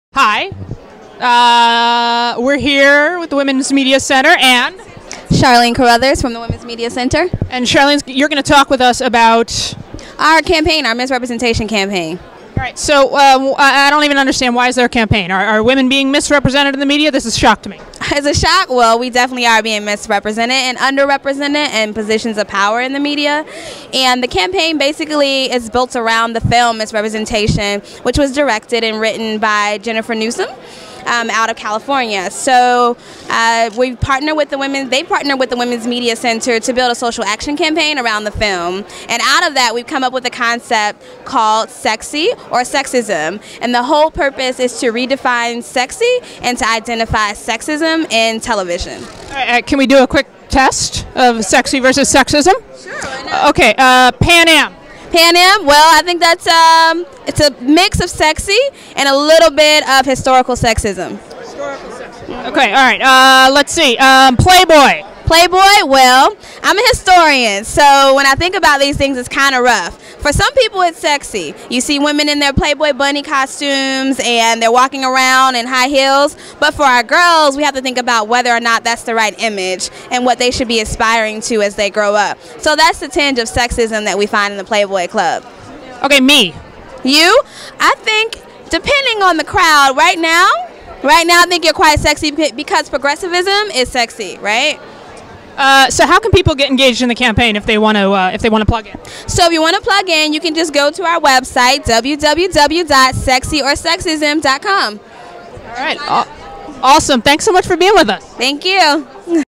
Charlene Carruthers, Women's Media Center
Sally Kohn interviews Charlene Carruthers from the Women's Media Center about Miss Representation.